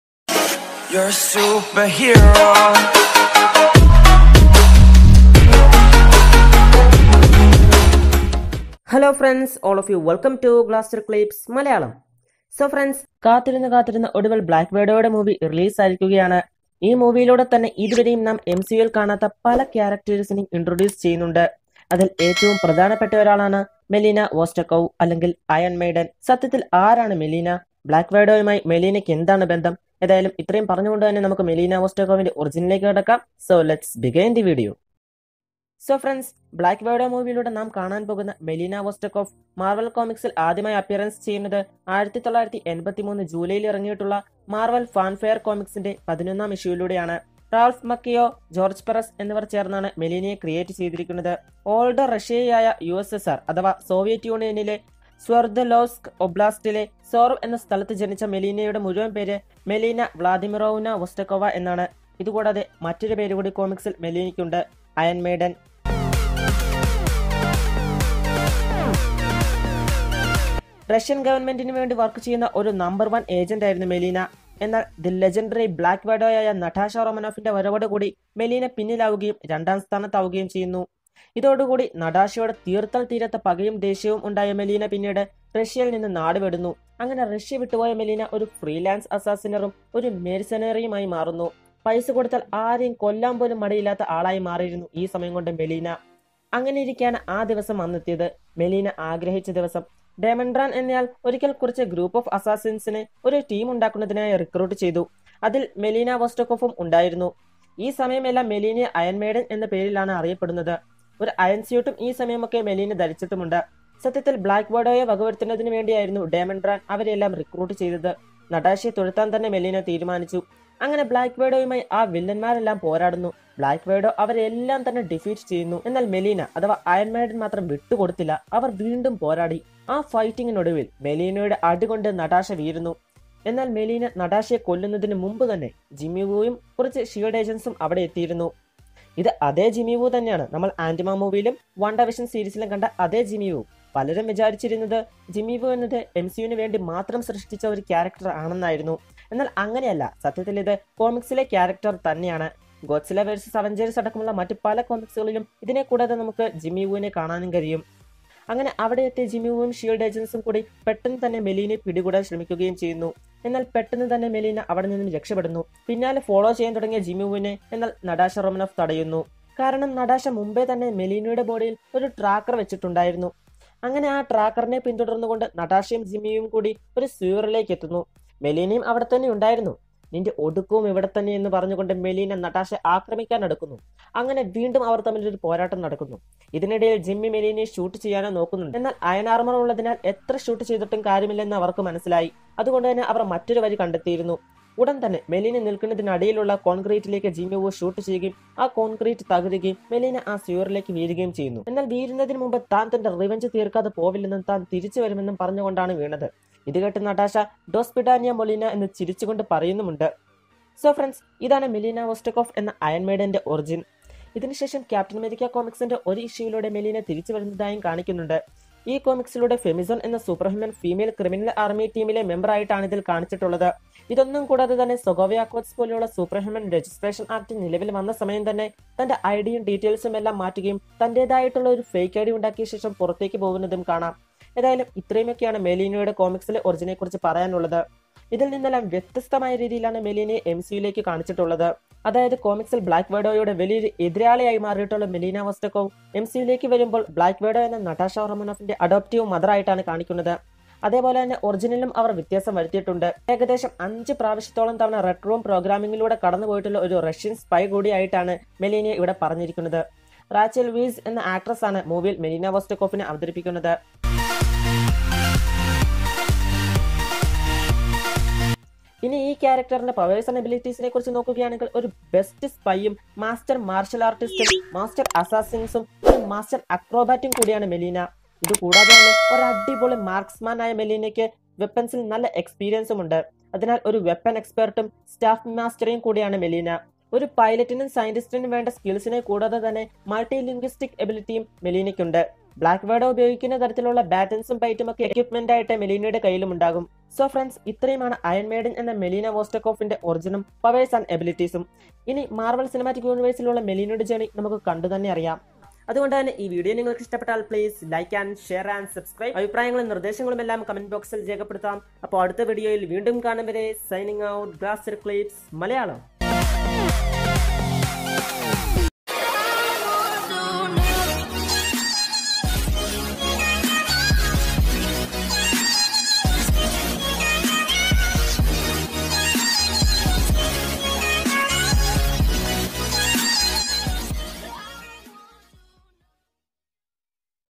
Melina Vostakoff/Iron Maiden Origins Powers & Abilities | Glaster Clips Malayalam
Hlo frnds.

In this video, I am talking about the new leaks of the origins and powers and abilities of oron maiden aka melina vostakoff in marvel comics and mcu introduced in black widow movie.